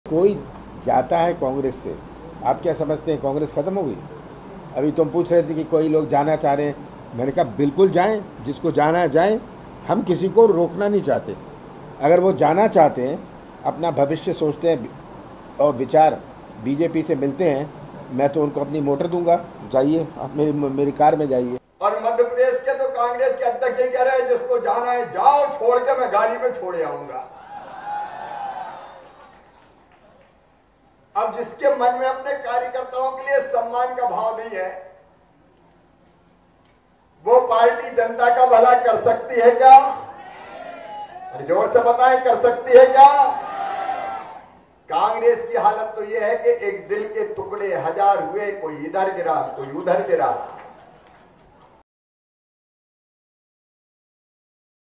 CM Shivraj Singh ने Kamal Nath के बयान पर ली चुटकी
Kamal Nath Madhya Pradesh के पूर्व CM हैं जिन्हुनें हाल ही में एक बयान दिया था और बोला था जिसे BJP ज्वाइन करनी है वो करे. इस पर मुख्यमंत्री Shivraj Singh Chouhan ने क्या कहा है आइये सुनते हैं.